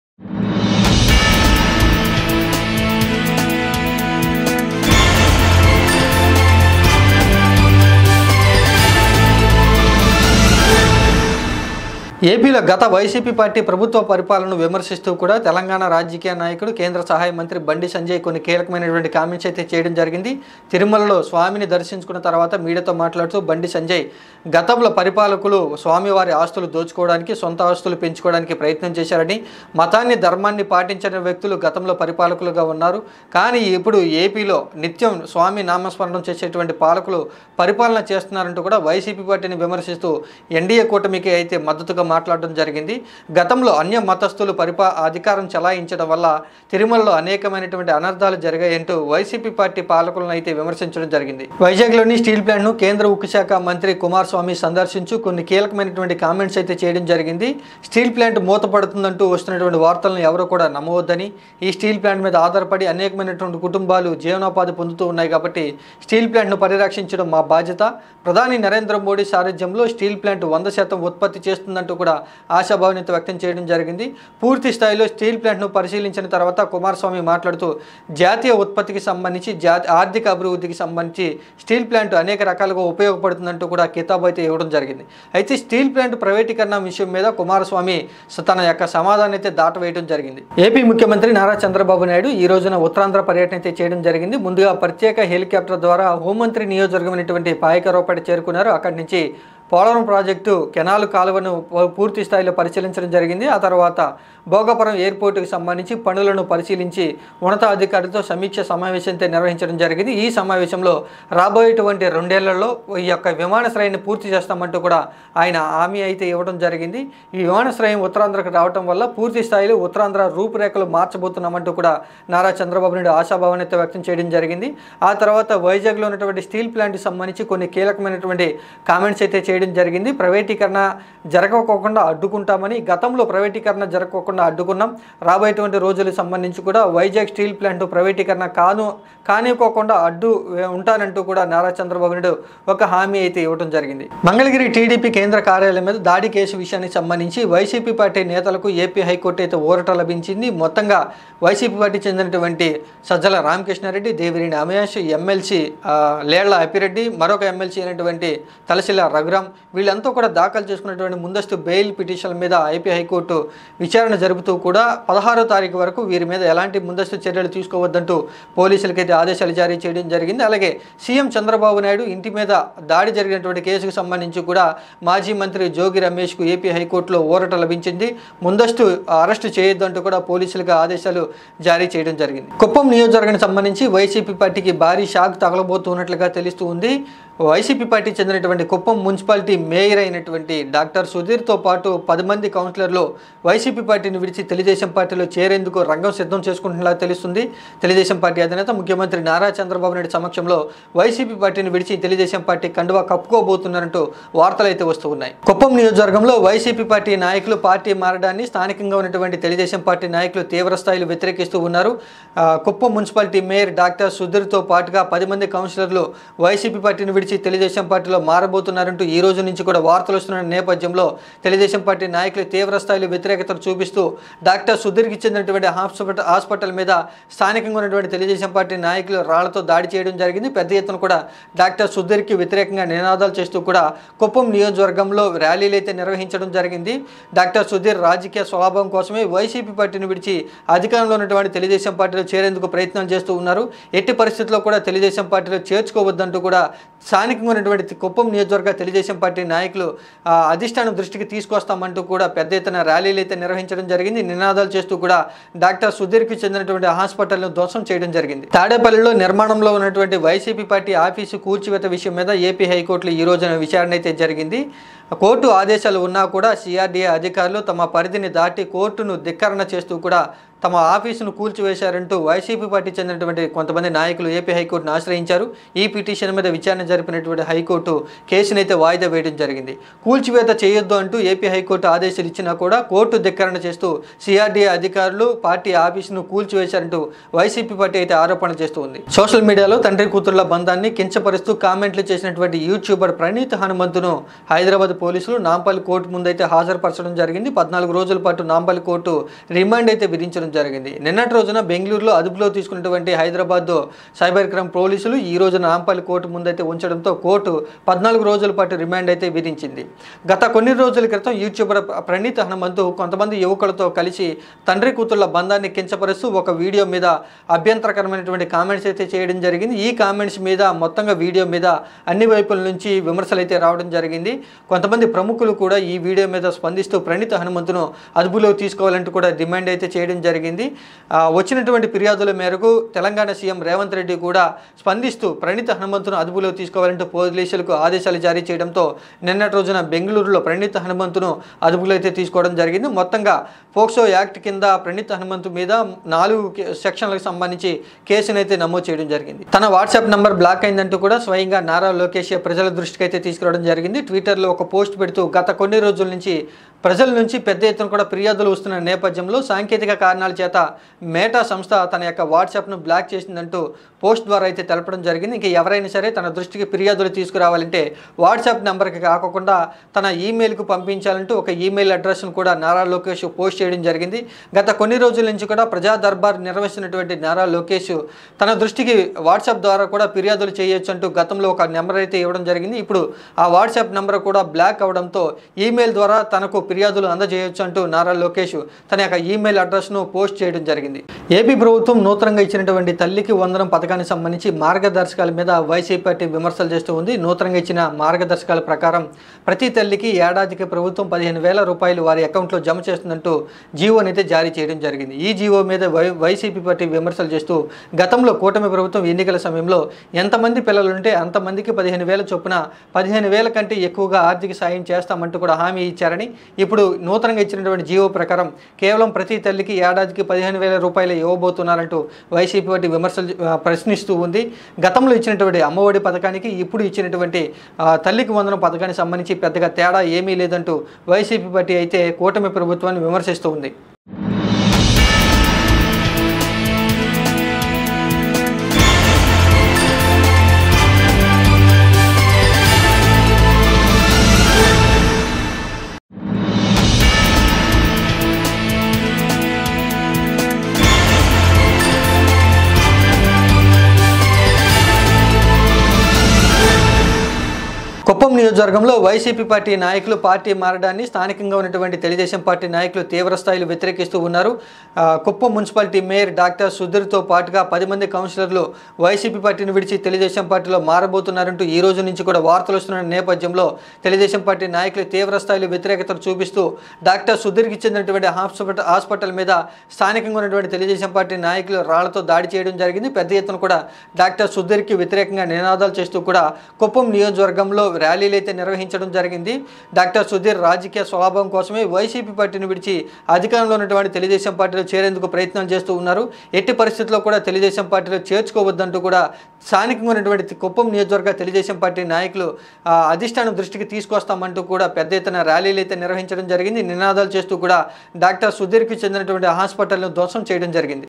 Bandi Sanjay Sensational Comments On YS Jagan : Today Top 10 News : PDTV News
Bandi Sanjay Sensational Comments On YS Jagan : Today Top 10 News

Bandi Sanjay makes sensational comments on YS Jagan in today's top 10 news on PDTV News. Stay updated on all the latest news from Chandrababu, YSRCP, CMO, and more!